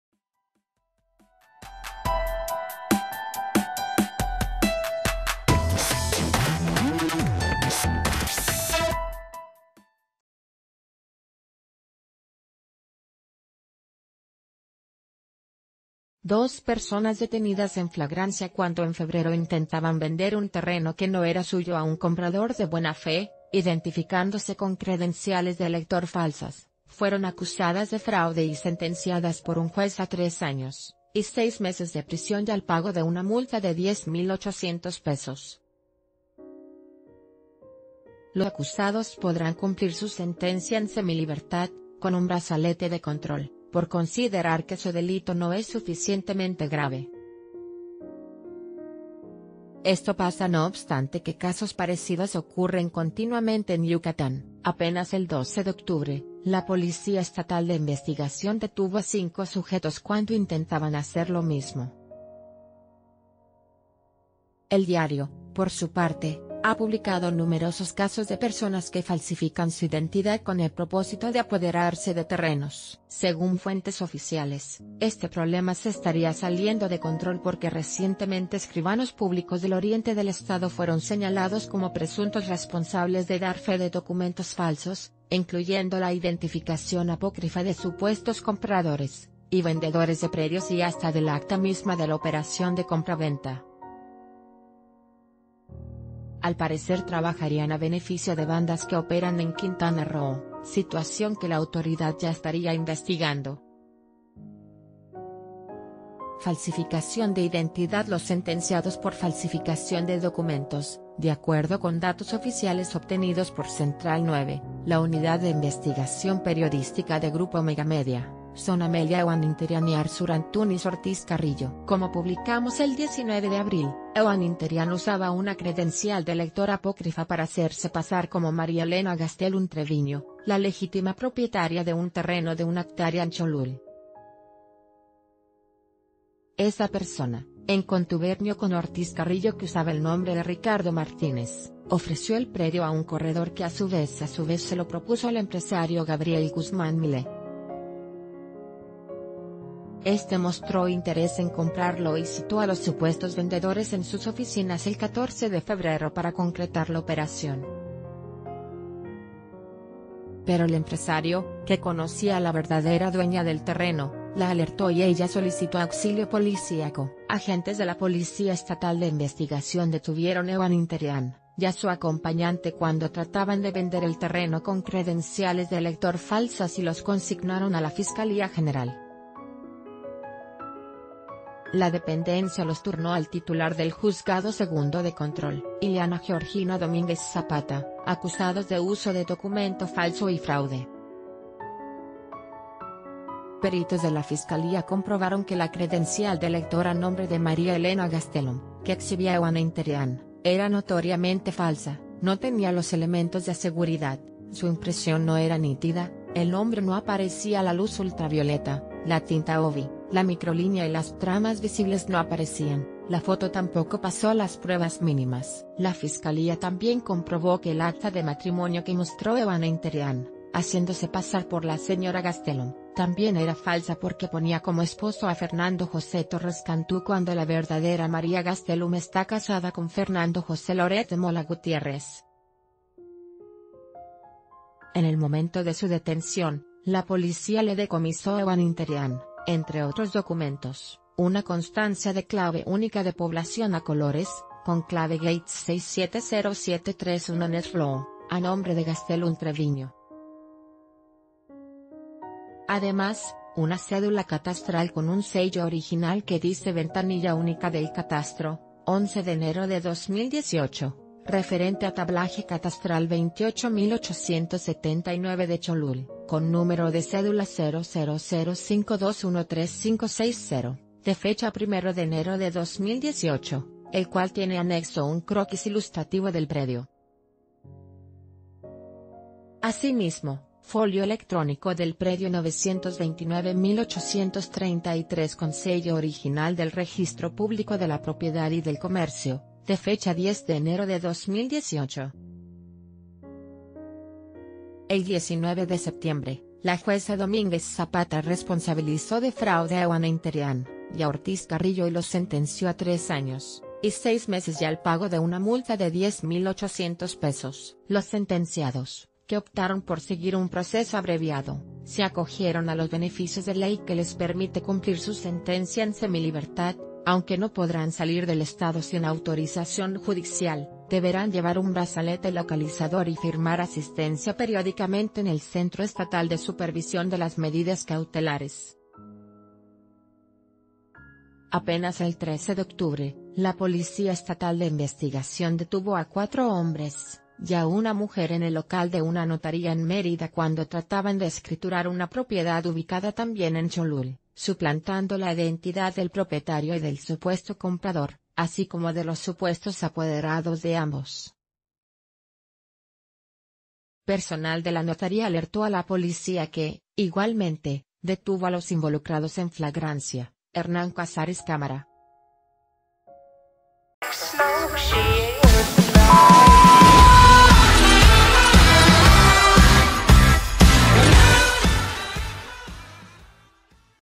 Sentenciados por fraude
Dos personas detenidas en flagrancia cuando en febrero intentaban vender un terreno que no era suyo a un comprador de buena fe, identificándose con credenciales de elector falsas, fueron acusadas de fraude y sentenciadas por un juez a tres años y seis meses de prisión y al pago de una multa de 10,80...